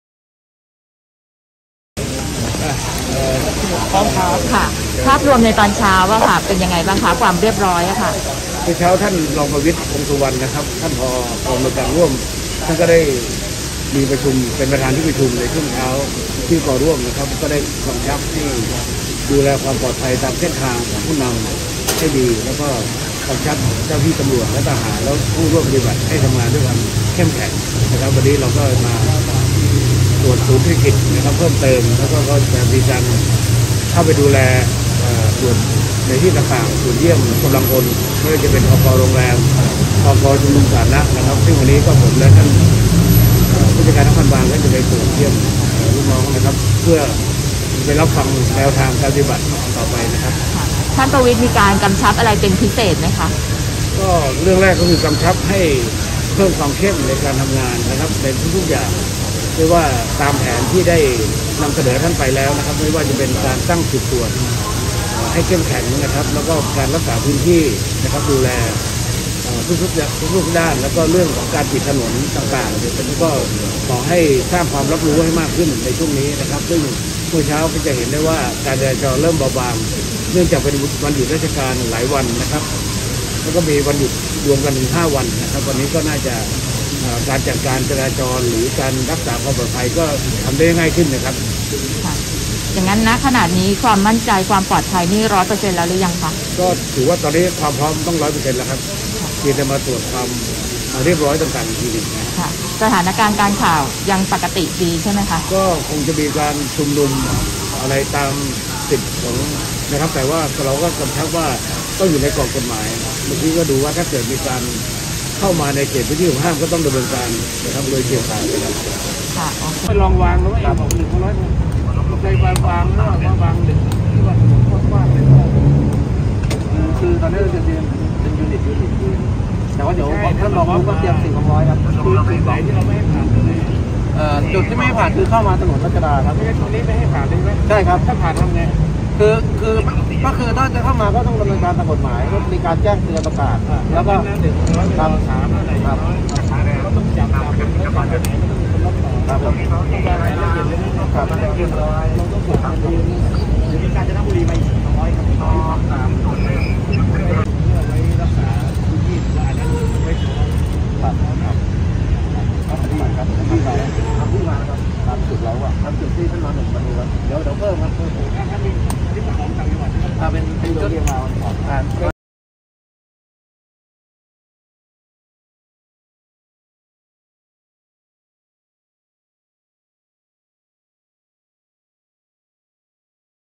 เข้มจุดคัดกรอง ผบ.ตร. ตรวจพื้นที่ มั่นใจดูแลความปลอดภัย 100% รับเอเปค
พลตำรวจเอกดำรงศักดิ์ กิตติประภัสร์  ผู้บัญชาการตำรวจแห่งชาติ พร้อมชุดดูแลความปลอดภัย เข้าตรวจดูแลความเรียบร้อยกำลังพลโดยรอบศูนย์ประชุม โดยระบุว่า ขณะนี้มั่นใจการดูแลความปลอดภัยเต็ม 100%  จากการที่ได้ลงสำรวจตรวจทุกจุด ภาพรวมเป็นไปด้วยความเรียบร้อยยังไม่พบสิ่งผิดปกติใดๆ

ทั้งนี้ จากการประชุมร่วมกับพลเอก ประวิตร วงษ์สุวรรณ รองนายกรัฐมนตรี ได้กำชับเรื่องเพิ่มความเข้มในการดูแลความปลอดภัยเส้นทางผู้นำ  เจ้าหน้าที่ตำรวจทหารที่ดูแลความปลอดภัยให้ทำงานด้วยความเข้มแข็ง และสร้างการรับรู้ให้กับประชาชนเรื่องการใช้เส้นทางให้มากขึ้น

ส่วนด้านการข่าวพบว่าจะมีการชุมนุมในบางจุด แต่ค้องอยู่ในกรอบของกฎหมาย หากมีการเข้ามามนพื้นที่หวงห้ามก็ต้องดำเนินการตามกฎหมาย